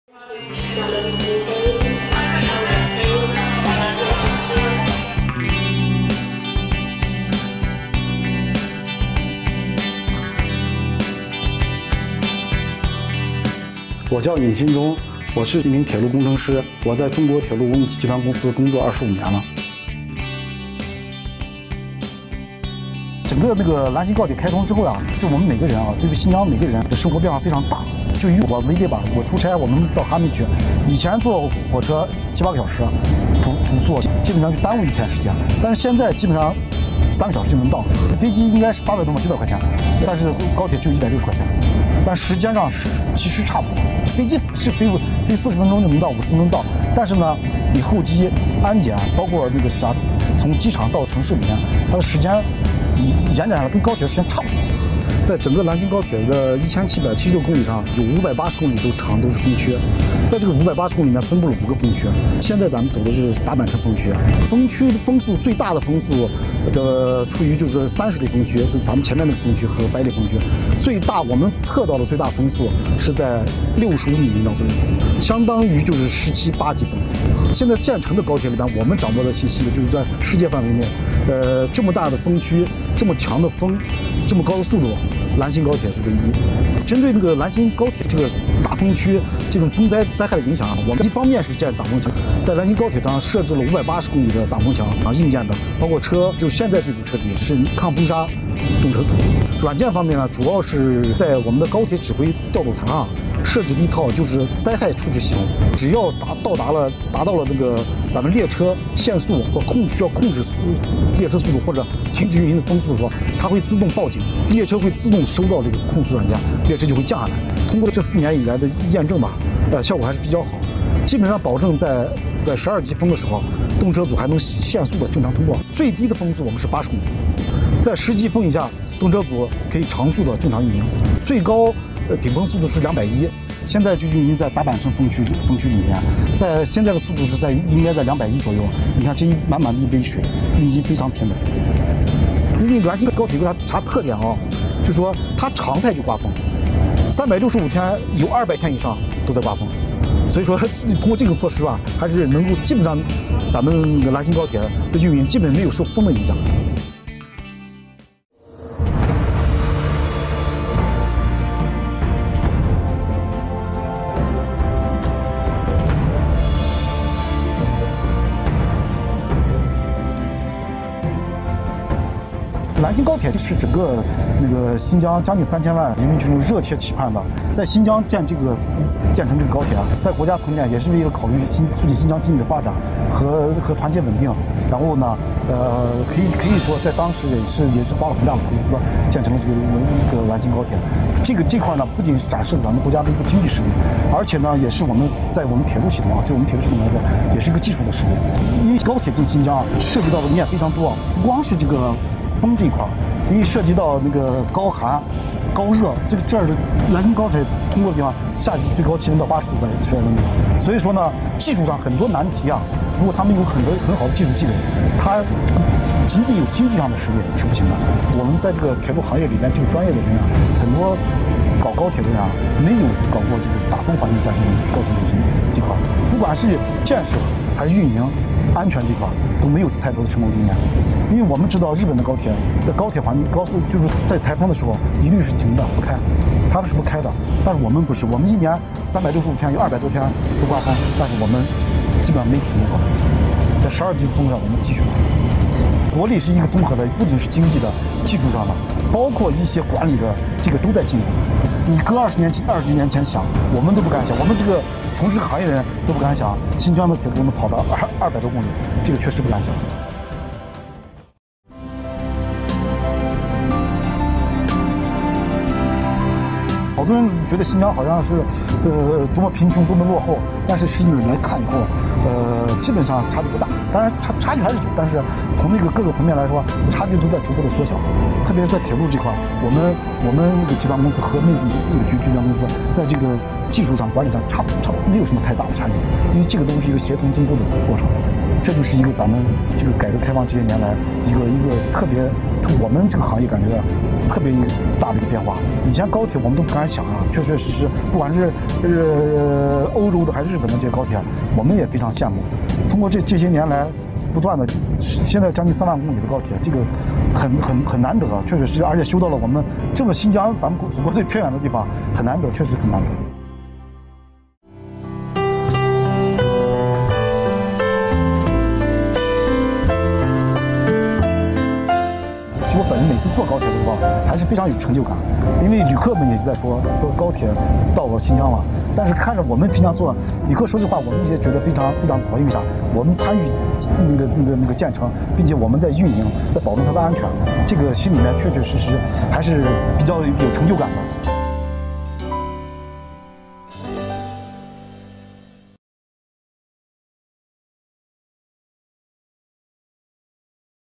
High speed railway speeds up Xinjiang development
The Lanzhou-Xinjiang high-speed railway is the first long-distance high-speed line in China's western region connecting Lanzhou, the capital city of northwest China's Gansu Province, with Urumqi, the capital of Xinjiang Uygur Autonomous Region. Since its operation began in 2014, the line has served more than 20.17 million passengers. It has expanded travel possibilities for residents along the line.